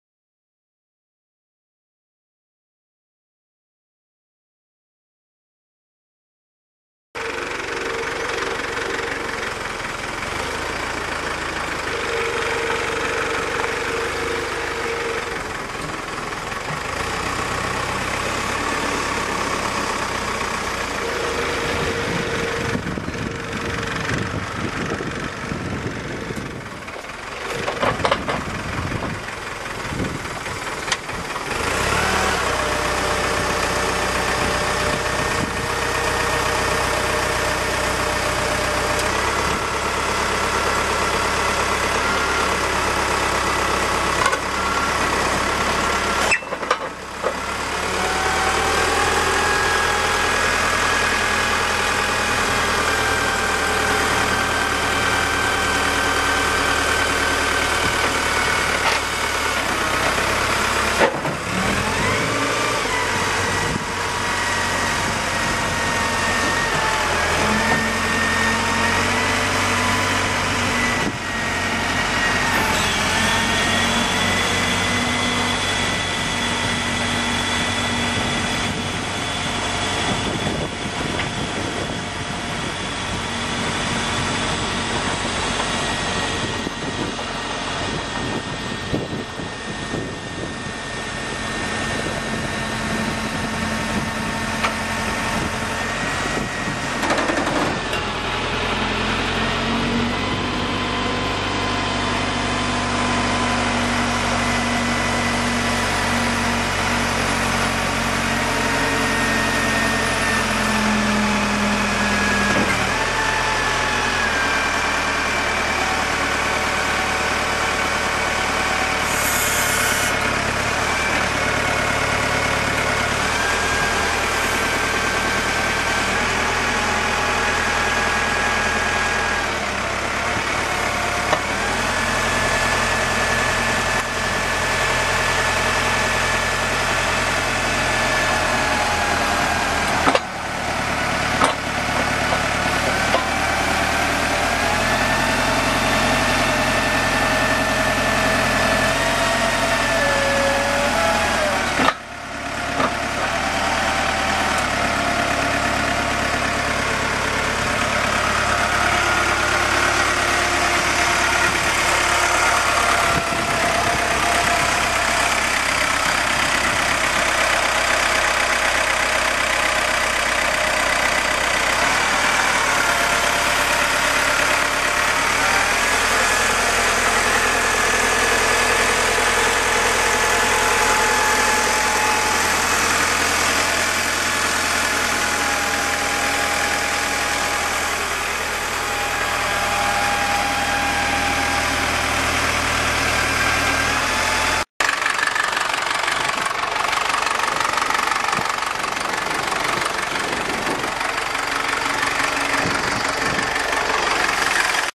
2004 Lull 1044C-54 telehandler for sale | sold at auction June 28, 2012
2004 Lull 1044C-54 telehandler
3,628 hours on meter
Cummins 4.5L four cylinder turbo diesel engine
Powershift transmission
4F-3R gears
Four wheel steer
Hydraulic outriggers
Hydraulic body tilt
54&#039; maximum lift height
10,000 lb lift capacity
48" fork length
Side tilt carriage
Work lights
14.00-24 tires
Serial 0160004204

It was located in Norman, OK, and we sold it to the highest bidder for $34,650.